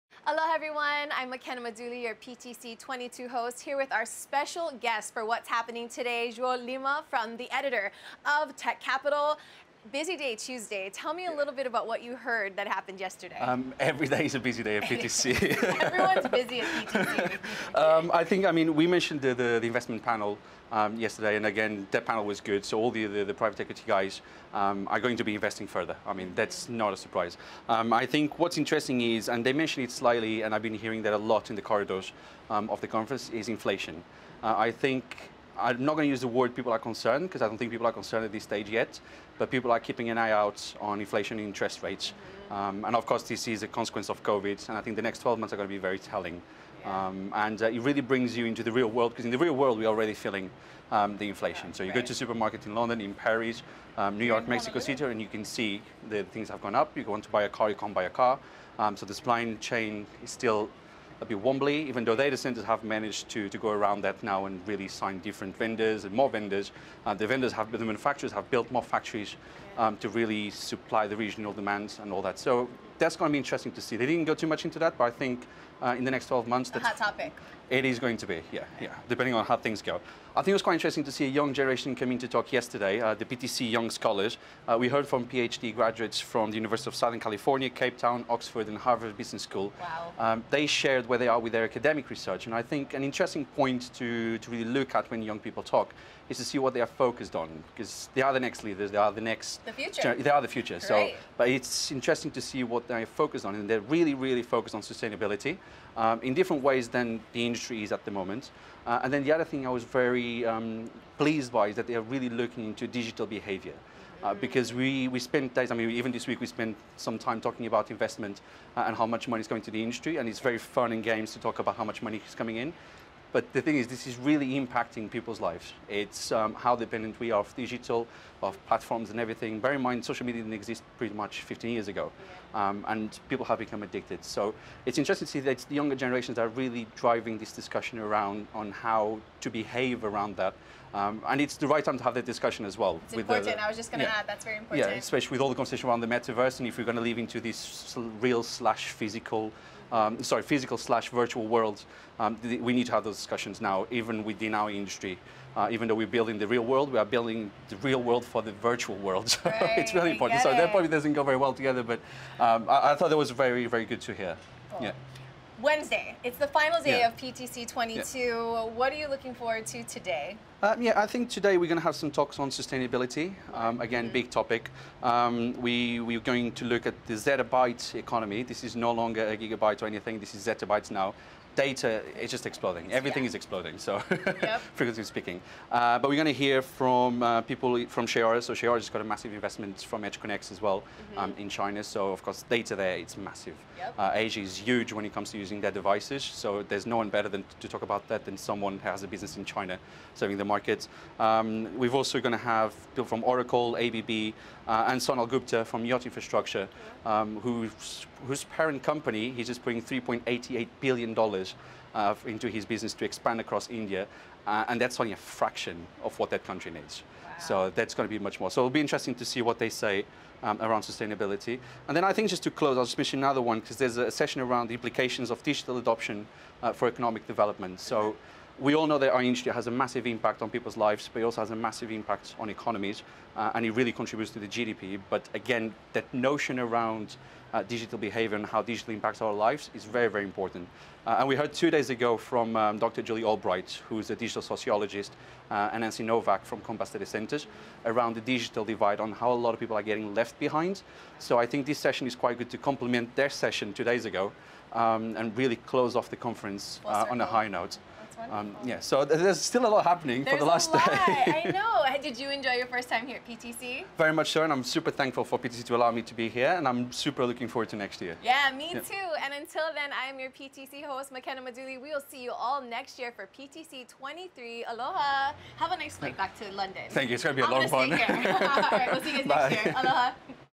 What's Happening at PTC'22 | Wednesday, 19 January 2022
A final discussion with João Marques Lima about his highlights from the Tuesday program at PTC'22, and what he's looking forward on Wednesday, 19 January 2022.